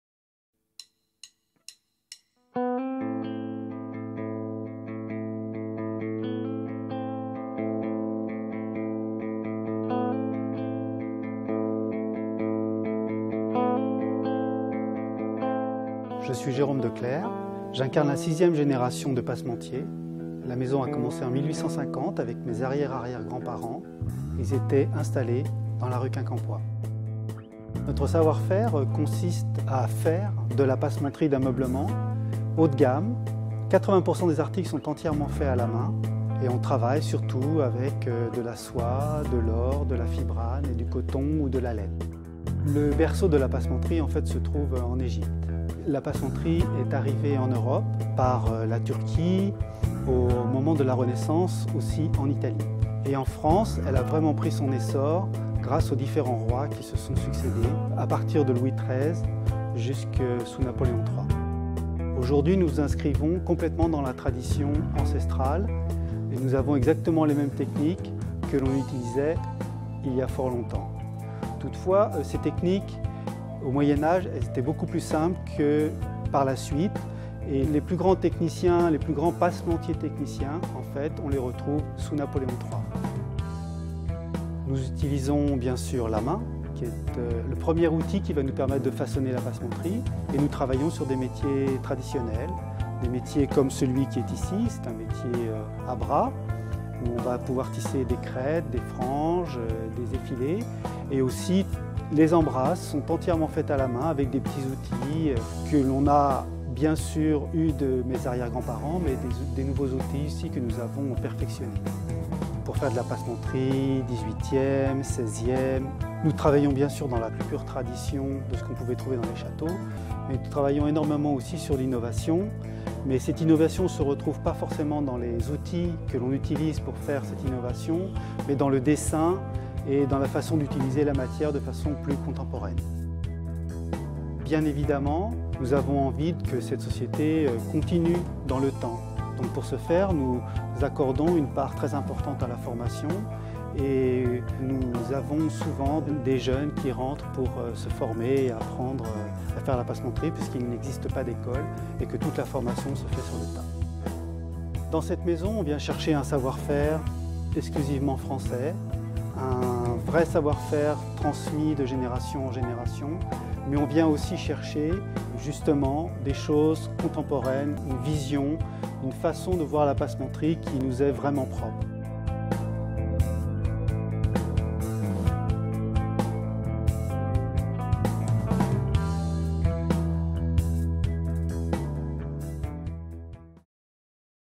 Trophée Trésor Vivant de l'Artisanat - Declercq Passementiers
Jérôme Declercq a été nominé pour le trophée Trésor Vivant de l'Artisanat.